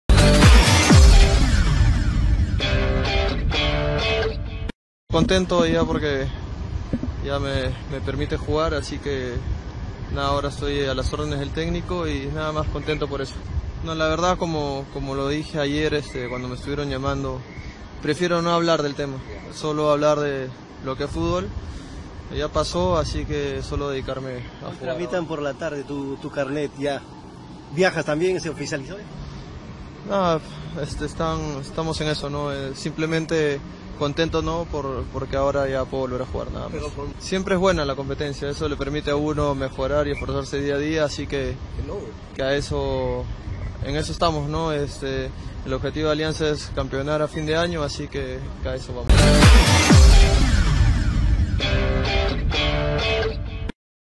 Mauricio Montes: "El objetivo es campeonar a fin de año".
Video y Edición: Info Blanquiazul